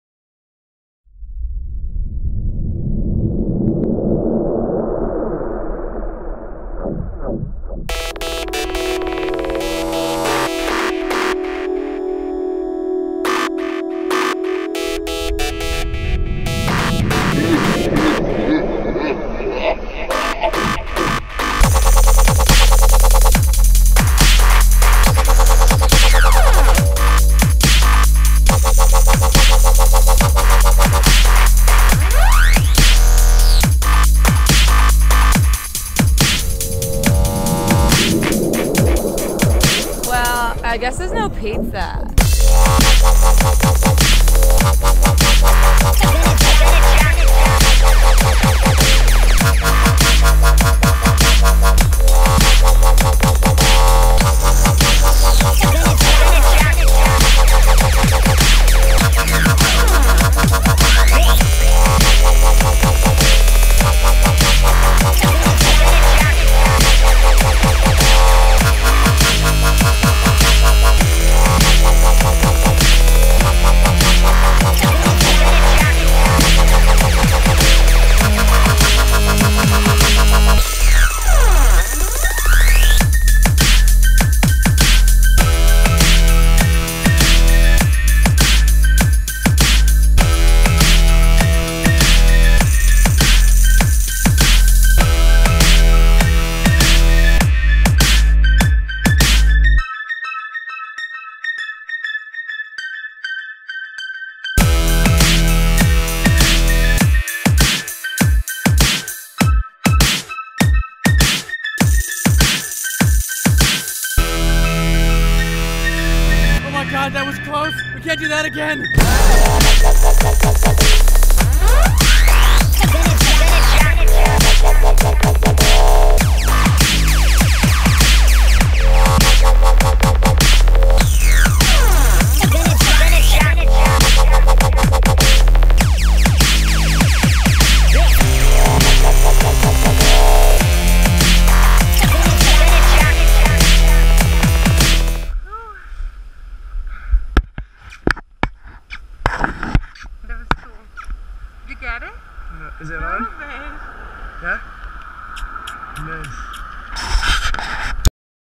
Kissin', Rubbin' and Bumpin'
wing walking and other shenanigans at Mentone.
Learn to fly with NeverLandParagliding.com